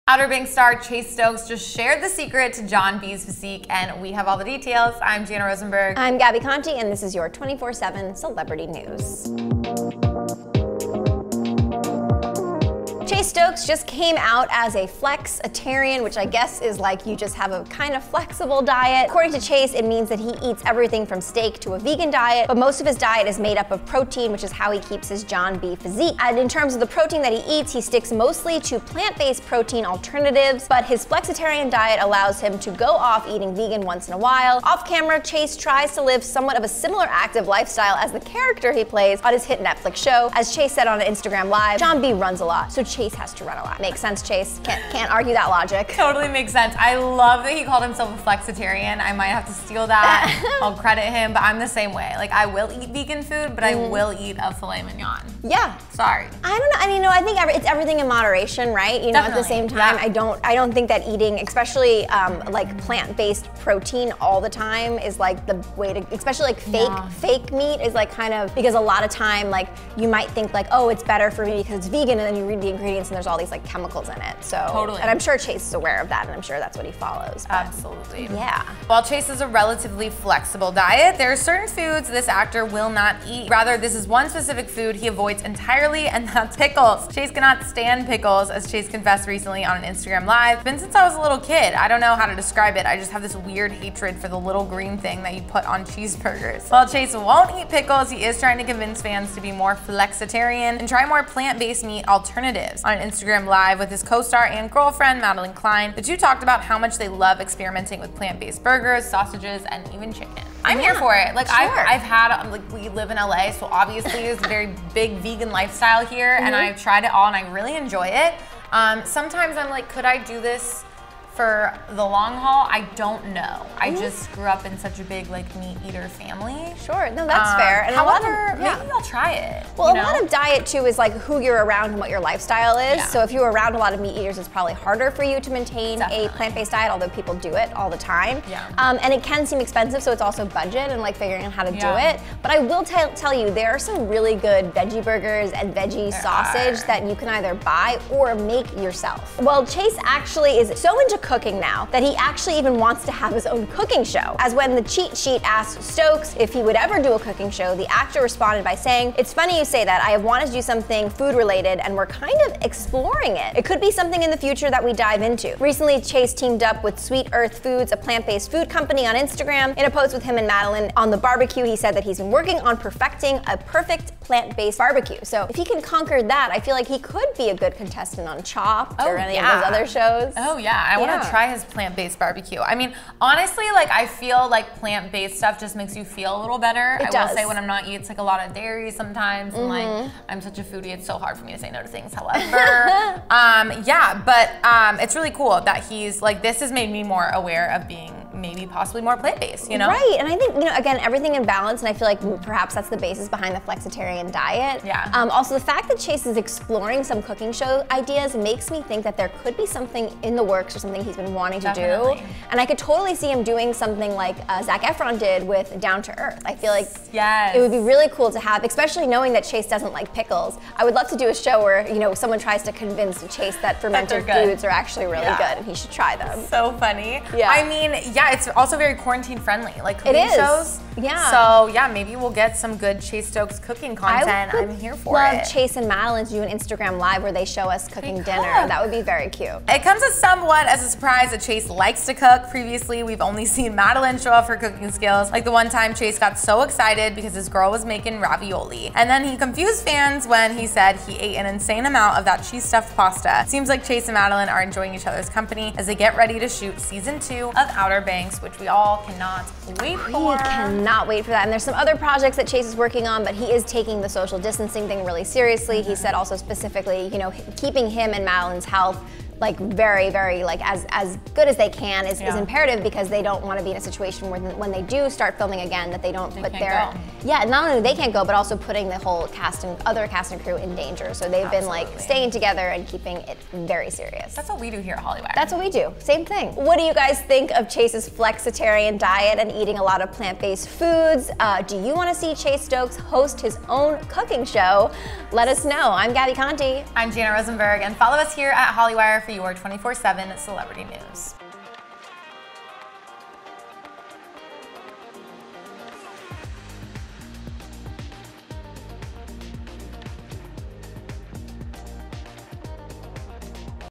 Chase Stokes Reveals Secret To His John B. Physique! | Hollywire
24/7 Celebrity News

Chase Stokes just shared the secret to John B's physique! He let fans know that he is a "flexitarian". Him and Madelyn Cline have been doing a lot of cooking during their time in quarantine together. Chase has been experimenting with different vegan recipes. Although, he is not going full vegan, he loves to incorporate vegan recipes into his meals! Hollywire's Jana Rosenberg and Gabi Conti have more details on his vegan cooking adventures and how he gets that perfect John B figure for 'Outer Banks'.